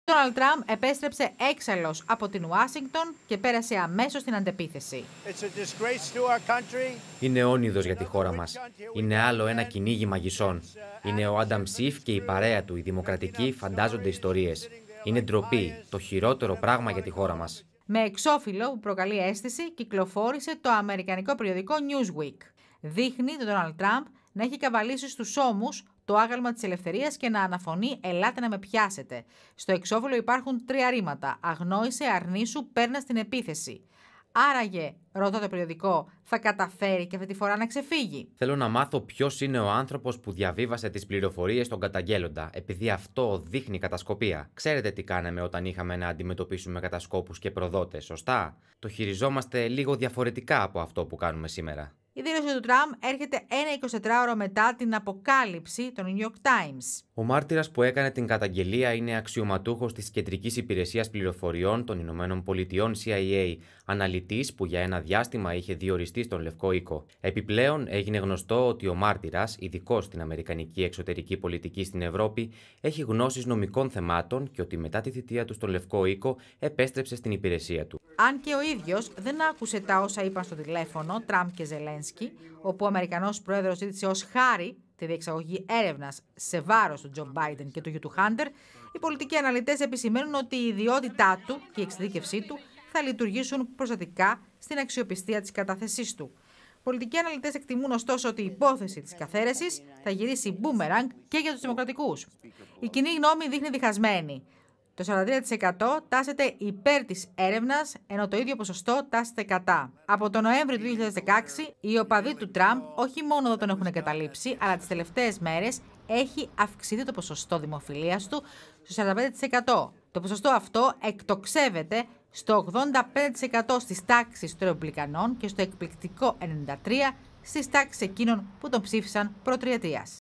ΗΠΑ: Στην αντεπίθεση ο Τραμπ - Τι λέει για τον πληροφοριοδότη
Ανεβαίνει το θερμόμετρο στην αμερικανική πολιτική σκηνή μετά την εκκίνηση της διαδικασίας καθαίρεσης του Αμερικανού προέδρου που αναμένεται να ξεκινήσουν οι Δημοκρατικοί. Ο Ντόναλντ Τραμπ, απείλησε εμμέσως πλην σαφώς με θανατική ποινή τον πληροφοριοδότη που έδωσε υλικό στον μάρτυρα δημοσίου συμφέροντος.Την ίδια στιγμή, πάντως, οι πολιτικοί αναλυτές εκτιμούν ότι αυτή η υπόθεση μπορεί να ευνοήσει επικοινωνιακά τον Αμερικανό πρόεδρο.